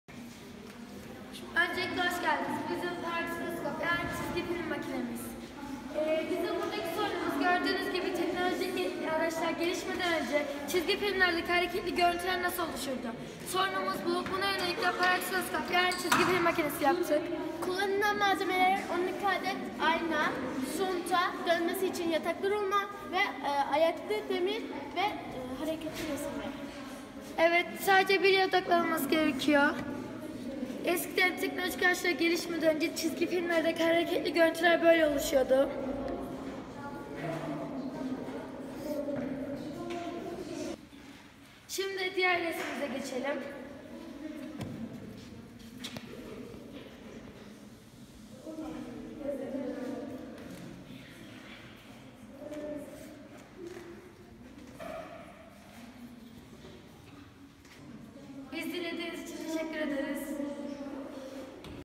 Eskiden çizgi filmler nasıl yapılıyordu.?
PROJE AMACI: Çizgi film canlı ya da cansız nesneleri canlandırmak ve onlara yeni bir kimlik verme işlemidir. Ancak çizgi filminyapılanma süreci birden olmamıştır. Çizgi filmin en önemli öğesi devinimdir. Bunun yanı sıra hızdan ve yinelenmeden yararlanır. Çizgifilm devam eden resimlerdir. Her kare resimdeki cisimler gösteri sırasında hareket duygusu verecek şekilde düzenlenir. Resim kareleripeş peşe oynatılarak animasyon elde edilir.1800 lü yıllarda İngiliz matematikçi William George Horner tarafından düşünülen bu sistemoniki yüzlü prizma şeklinde monte edilen aynalı bir optik denkleştirme aletidir. Emile Reynaud ise optik tiyatronun da yaratıcısıdır. Işıkizlenimlerinin retina üzerindeki sürekliliği sağlanarak hareket hissi veren bir alettir. Bu eğlenceli düzenek sinemanın sihirli dünyası içinbaşlangıçtır. Projemizin amacı çizgi filmlerin sırrını çözmektir ve matematiğin günlük hayattaki kullanımına örnek vermektir.